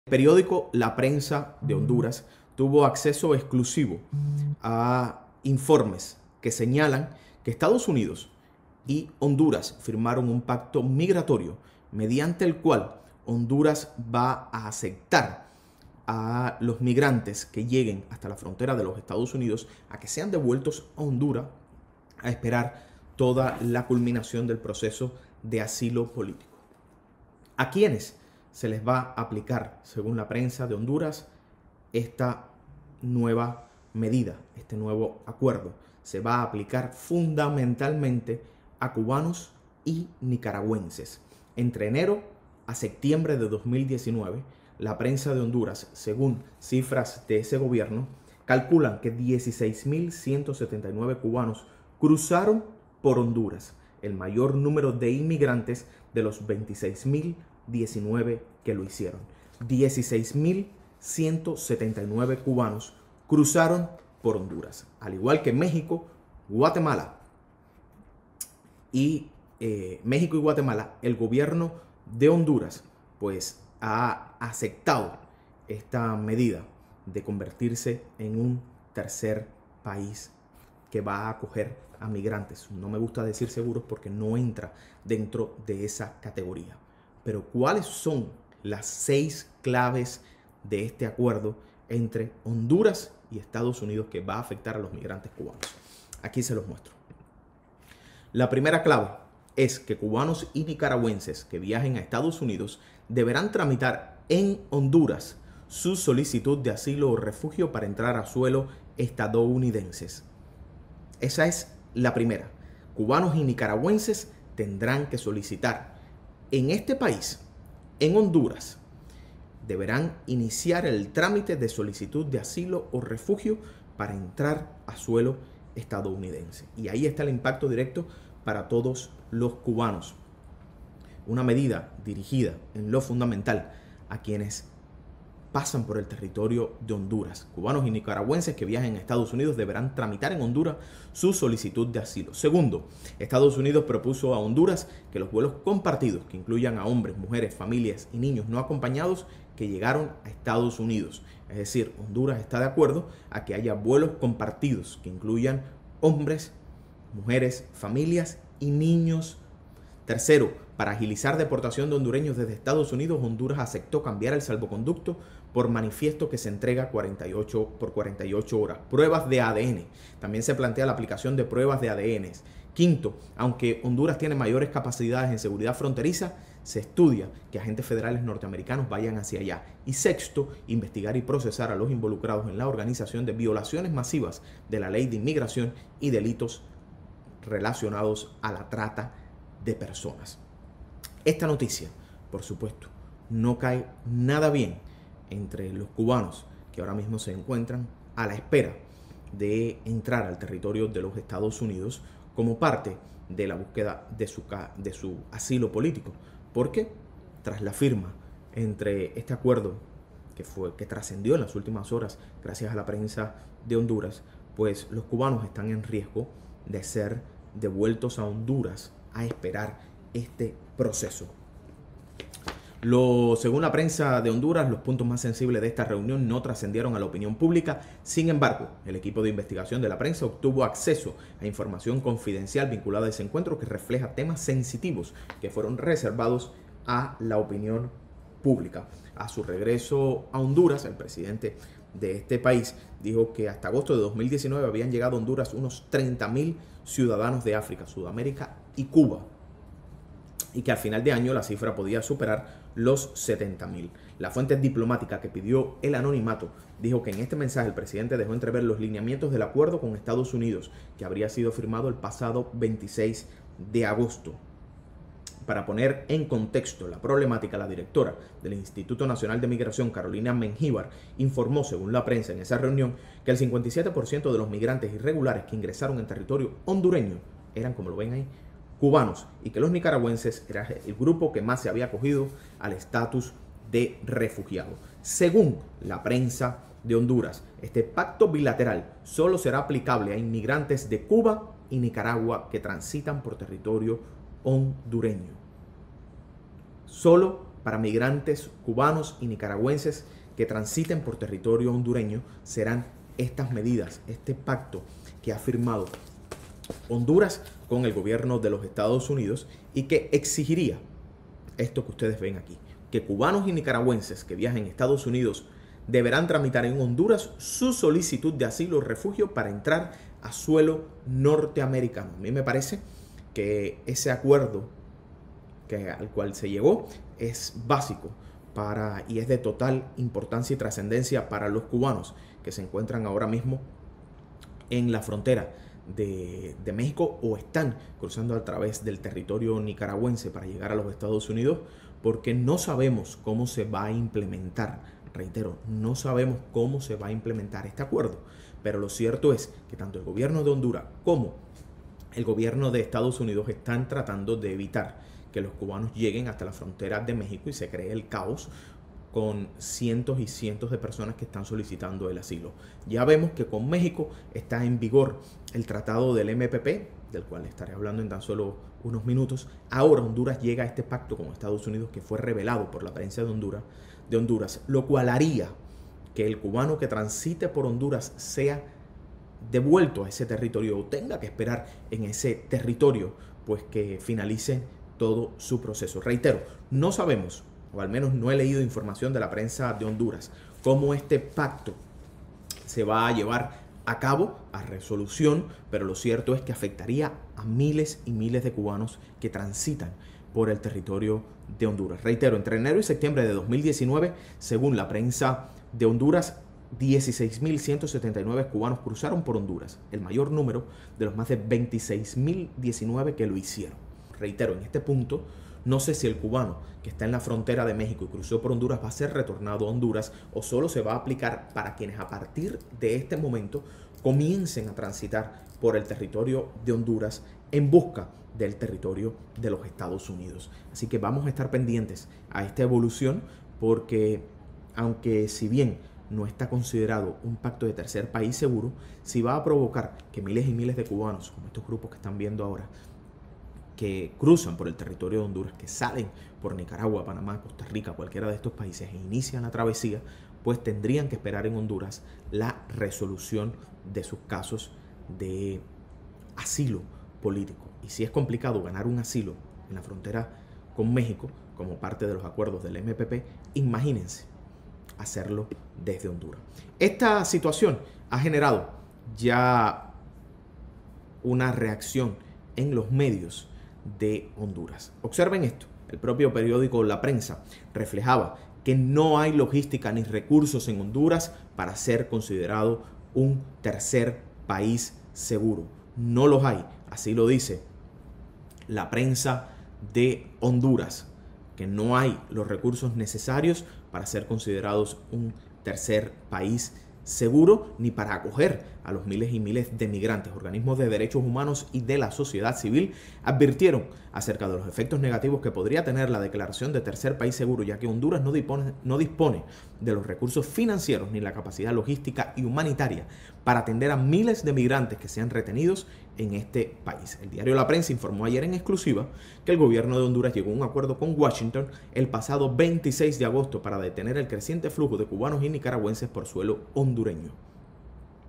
ATENCIÓN! HONDURAS LLEGA A ACUERDO CON ESTADOS UNIDOS QUE AFECTA A MIGRANTES CUBANOS Y NICARAGÜENSES
LA PRENSA, UNO DE LOS PERIÓDICOS MÁS IMPORTANTES DE CENTROAMÉRICA, EN UN REPORTAJE EXCLUSIVO SEÑALA QUE EL GOBIERNO DE HONDURAS LLEGÓ A UN ACUERDO CON ESTADOS UNIDOS PARA QUE LOS MIGRANTES CUBANOS Y NICARAGÜENSES QUE TRANSITEN POR SU TERRITORIO SE QUEDEN EN ESE PAÍS Y DESDE AHÍ INICEN SUS CASOS DE ASILO POLÍTICO. AQUÍ LOS DETALLES DEL REPORTE...